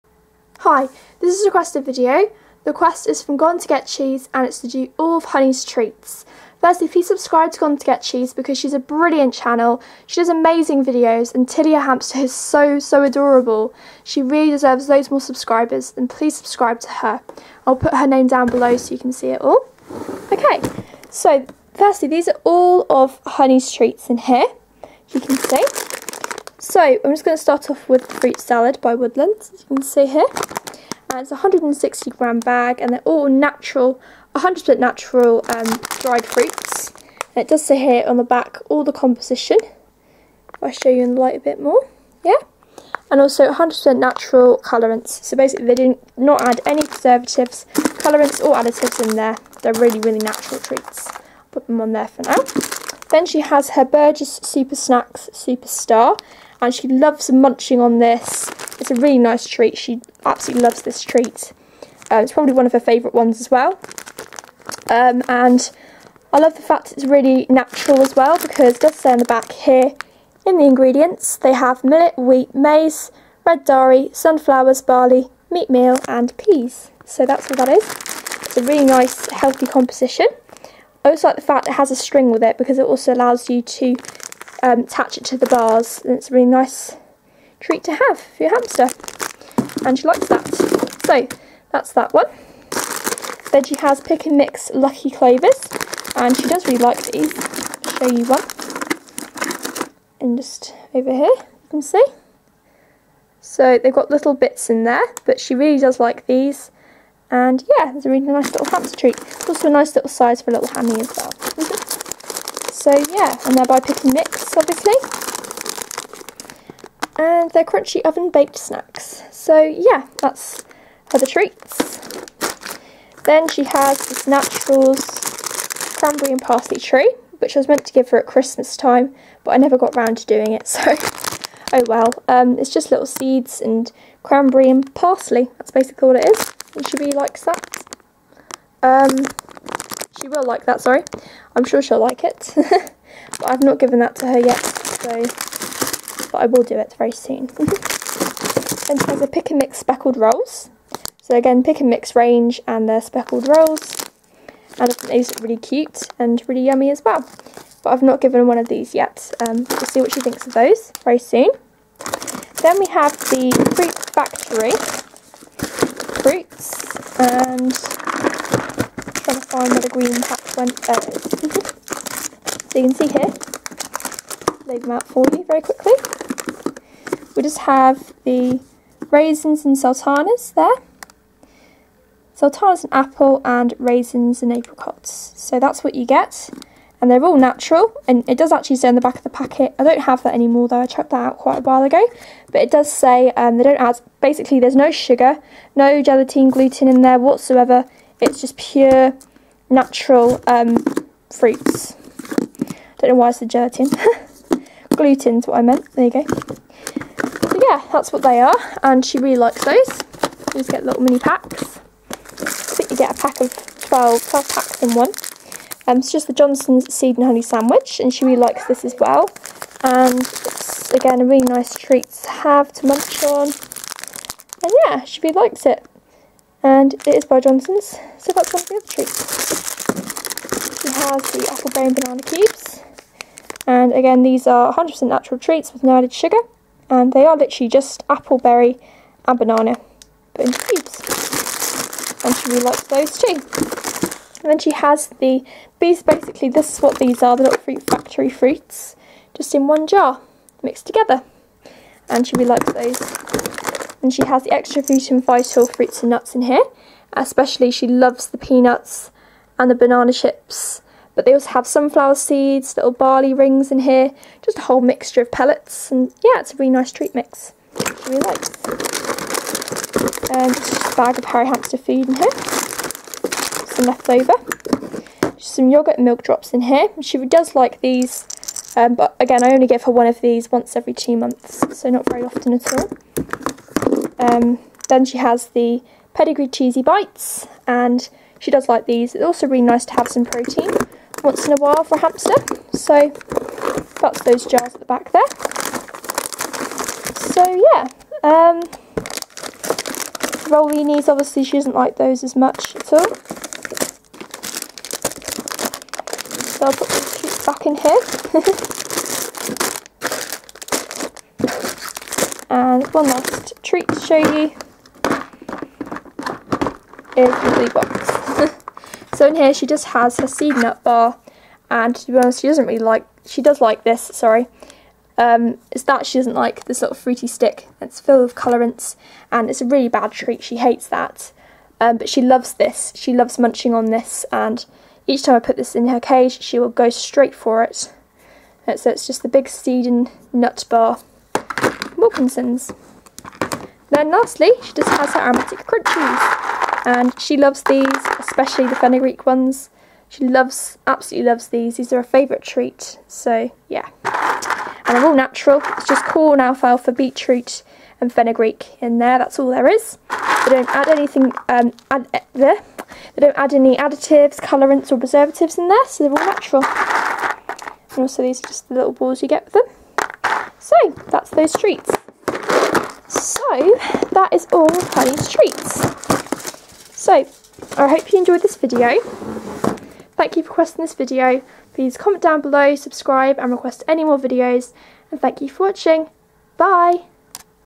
Hamster Treats (Request)
This is a requested video showing you all of Honey's treats. Sorry I rushed at the end of the video!!!